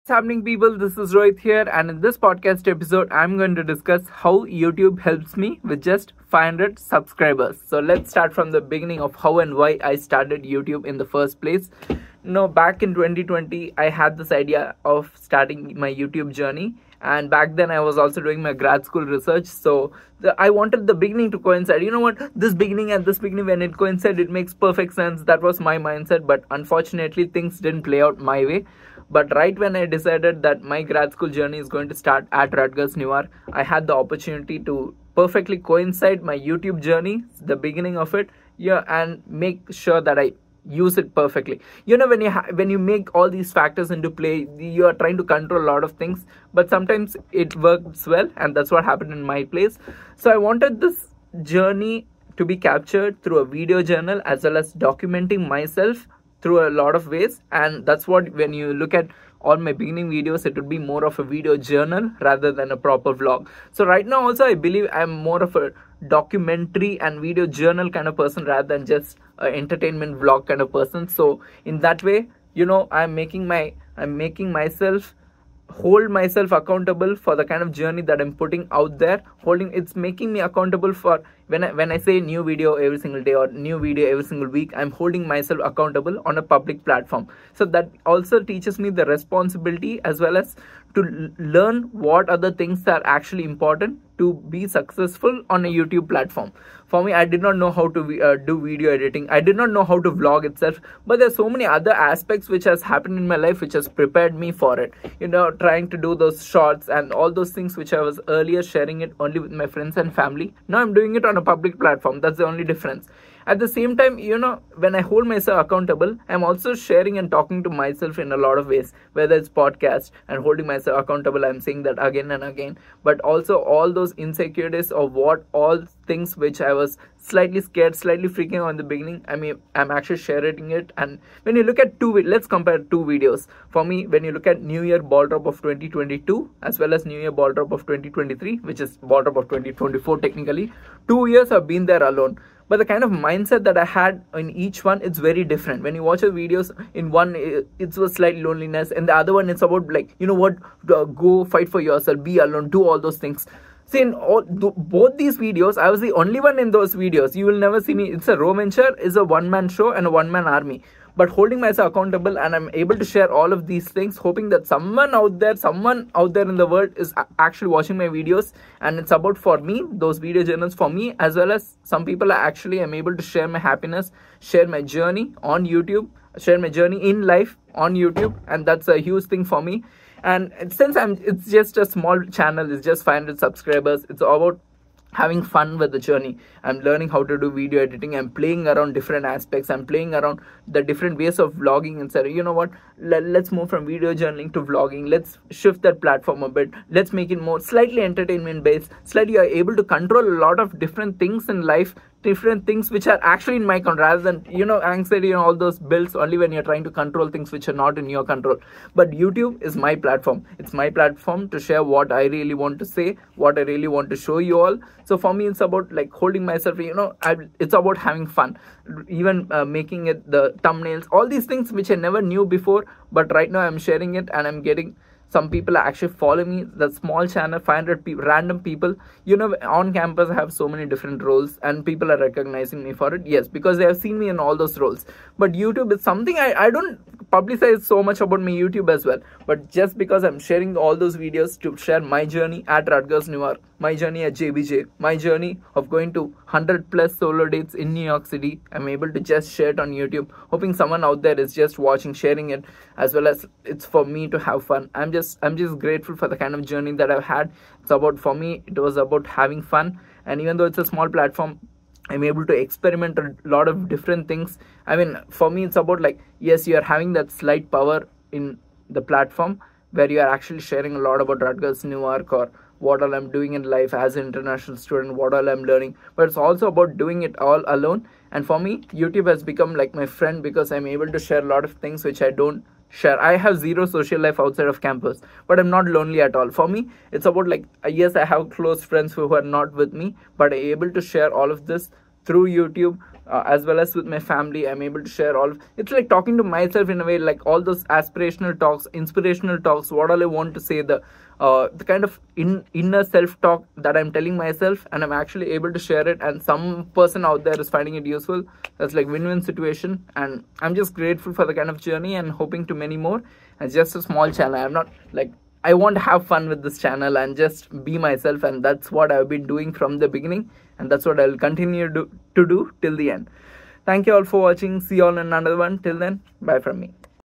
How YouTube Helps Me Grow
In this podcast episode, I discuss about how YouTube helps me with just 500 Subscribers

As an international student, I do video journals which offers me an unique perspective and stands as a witness to my growth. I get to document my journey online and hold myself accountable. It has helped me with my mental health a lot and I am grateful for it. I have learnt how to vlog, talk to camera and video editing only because of this channel.

I am also grateful to all my subscribers and y'all motivate me to do better. I love y'all. Thanks for being a part of my journey so far.